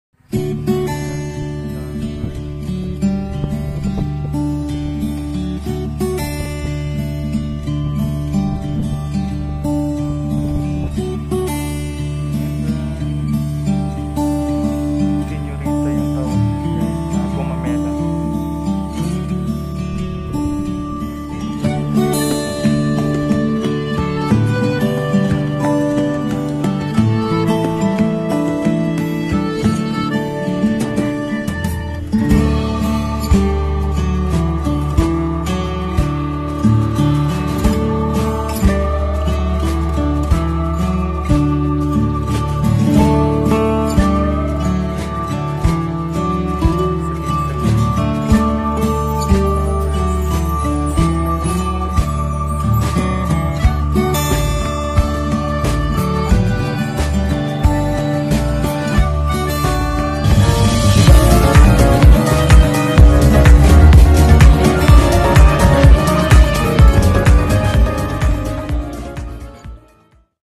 Pag Masdan Ang GanDa Ng Ibat IBAng Kulay Ng mga bulaklak
Nakakatuwa Pag masdan Ang GanDa Ng mga Bulaklak KAYa Tanim tanim tayo Pag may time We love you All GoD Bless you